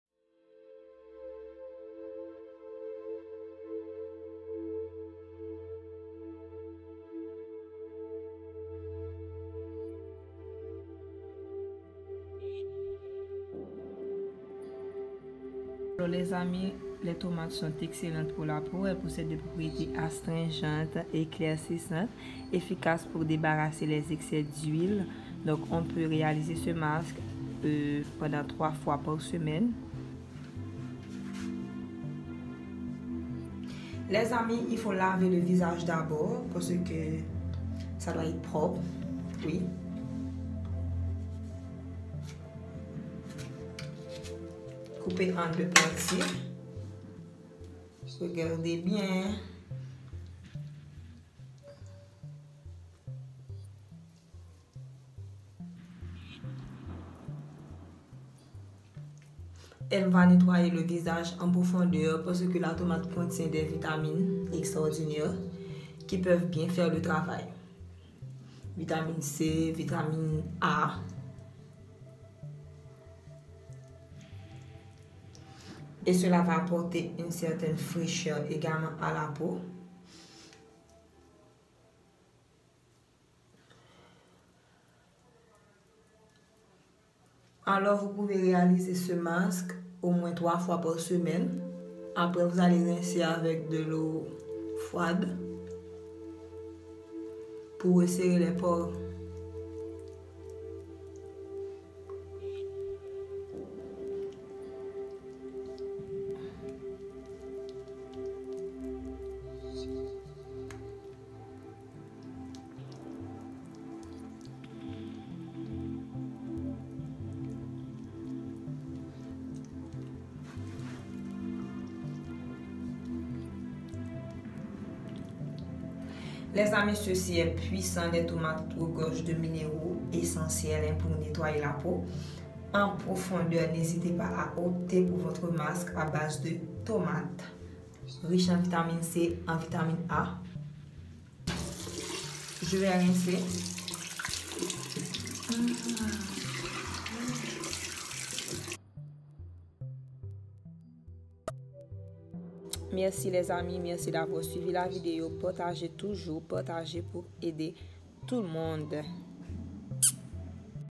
J’ai essayé ceci pendant une semaine et je suis choqué des résultats
Masque simple avec la tomate qui hydrate la peau ,élimine les boutons,lutte contre la peau terne.